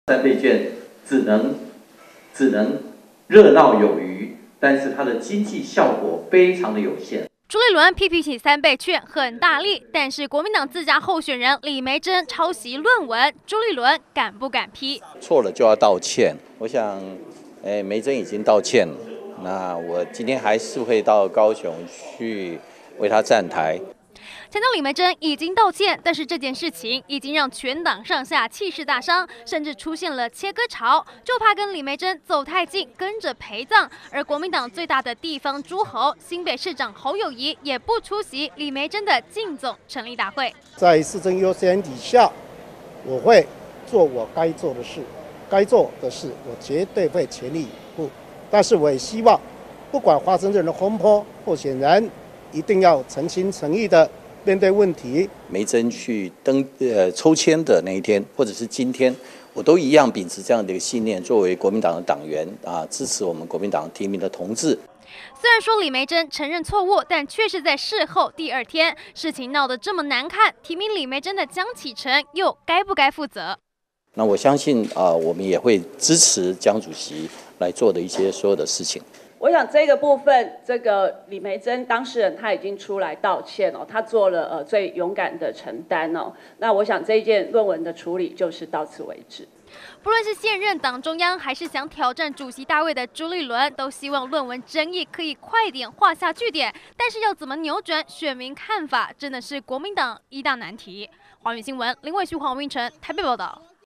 李眉蓁論文抄襲遭切割? 朱立倫:會挺她到底｜寰宇新聞20200725
【寰宇新聞】25日是國民黨高雄市長補選候選人李眉蓁的競選總部成立大會，但是因為李眉蓁的中山大學碩士論文抄襲爭議，國民黨內部出現不一樣的聲音，像是新北市長侯友宜就不克出席，基層也出現想與李眉蓁切割的聲音。不過前國民黨主席朱立倫表示，自己身為國民黨黨員，會挺李眉蓁挺到底，並認為李眉蓁已經勇敢坦承錯誤。